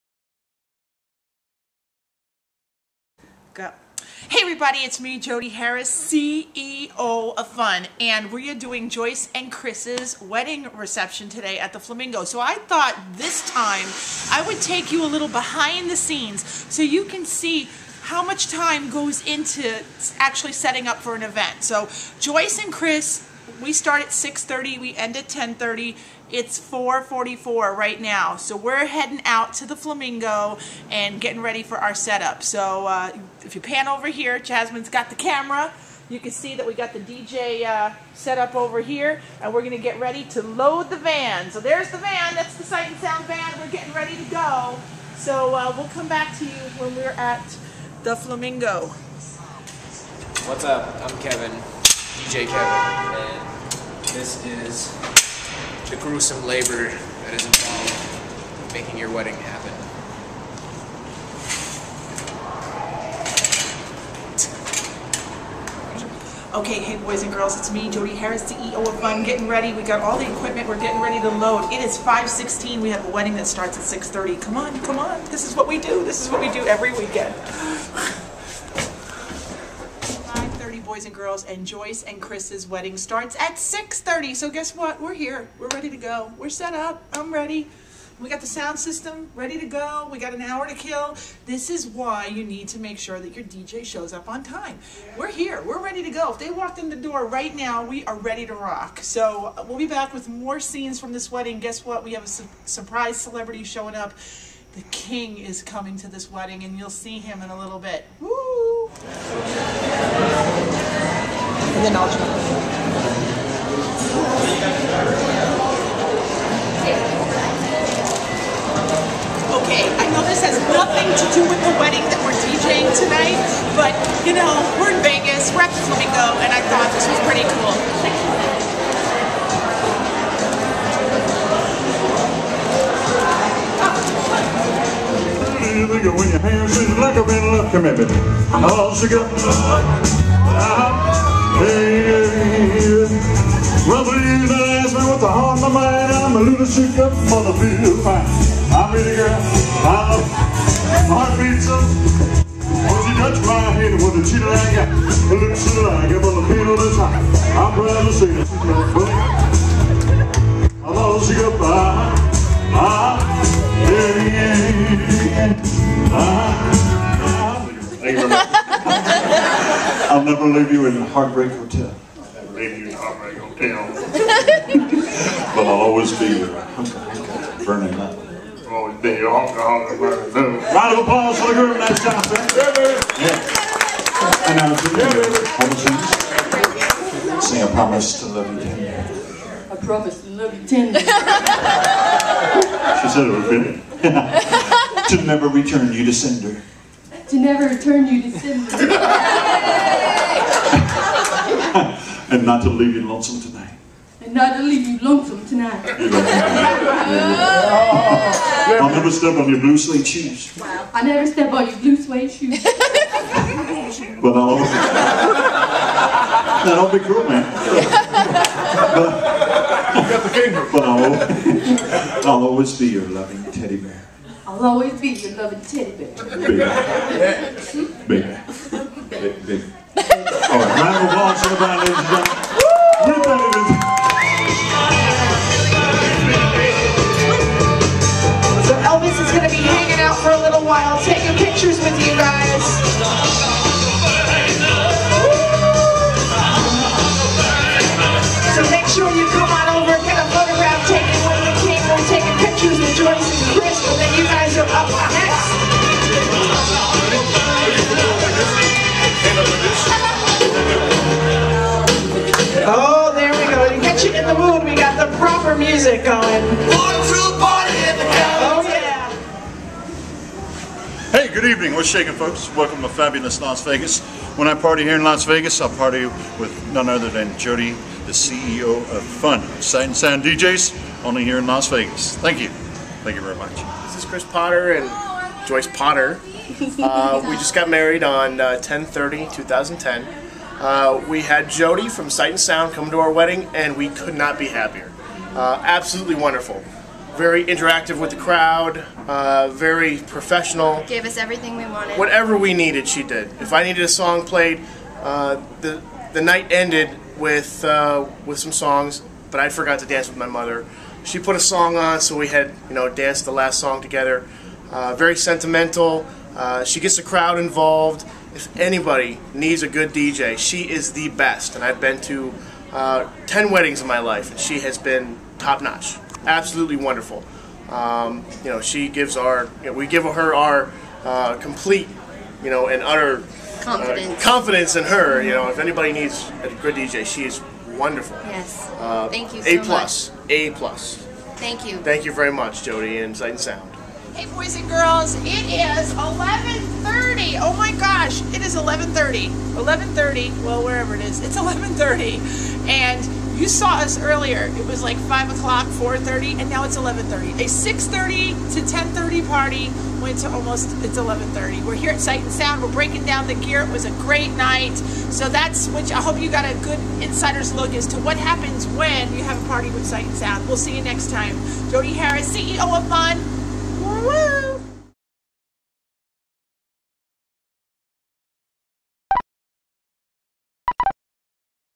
Flamingo Hotel & Casino Las Vegas | Wedding Testimonial
This is a BEHIND the scenes look into the making of a successful Sight & Sound Event.  Follow the CEO of FUN, Jodi Harris, as she takes you though a rare never before behind the scenes look into what goes into the making of a fun Las Vegas wedding reception for newlyweds, Chris & Joyce married at the Flamingo, Las Vegas.  You'll even see Elvis!